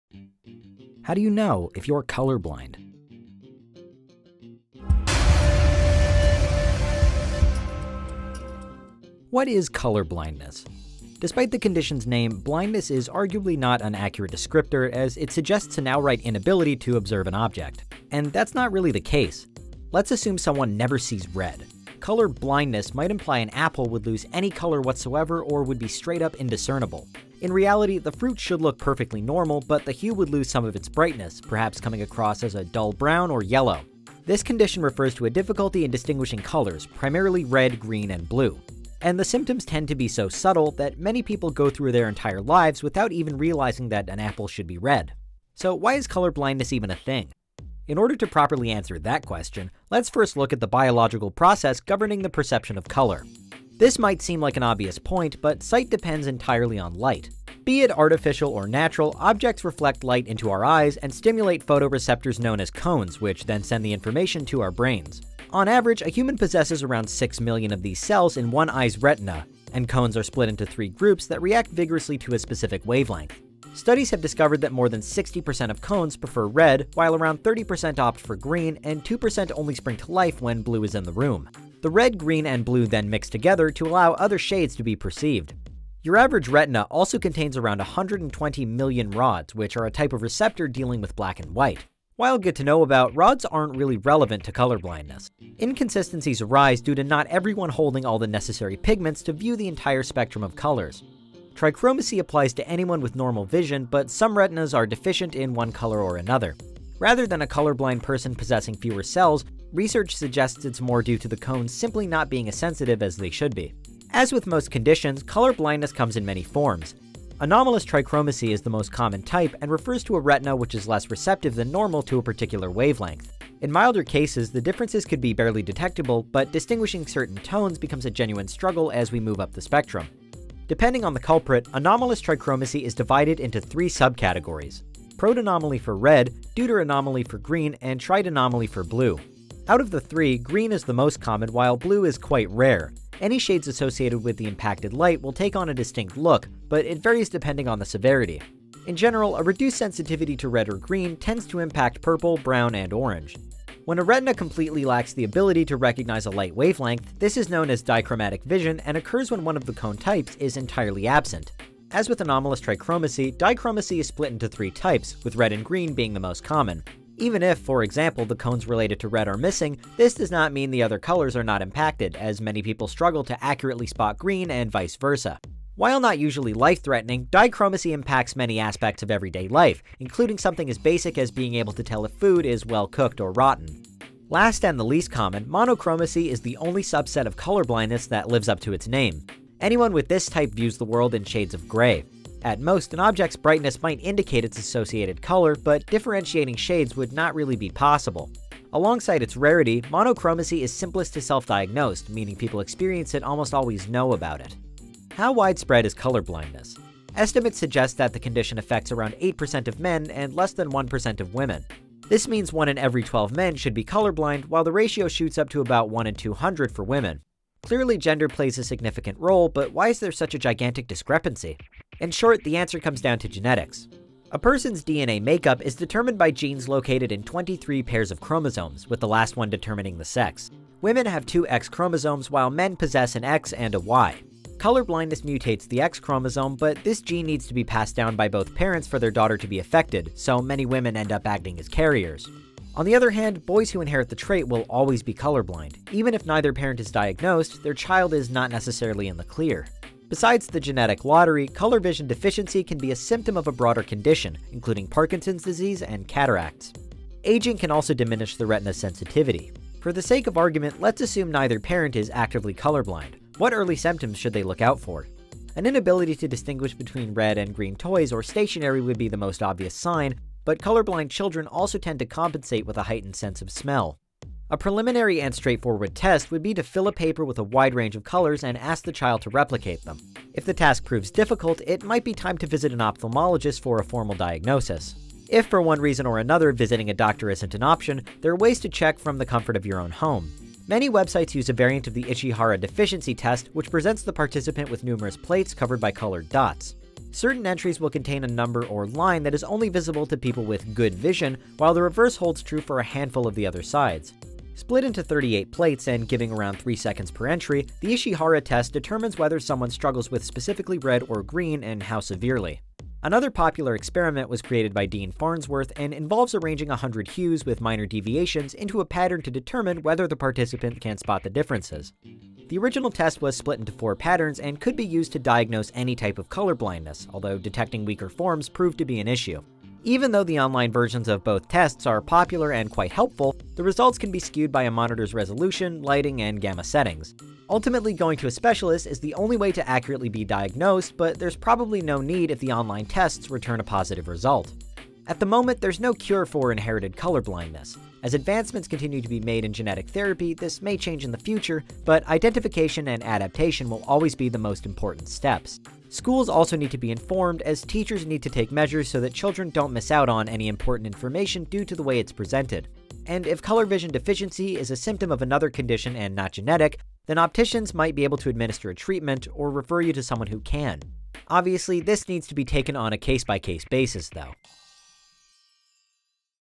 How Do You Know If You're Color Blind? | Unveiled
Millions of people around the world are color blind. Are you one of them? While color blindness presents itself in various different ways, all types of the condition are defined by one thing - a mixing of some colors, and a complete inability to perceive others. It's all caused by optic abnormalities within your eye, which mean that your brain process light differently. You can be born with color blindness, or it can develop over time - but it's typically very difficult to diagnose.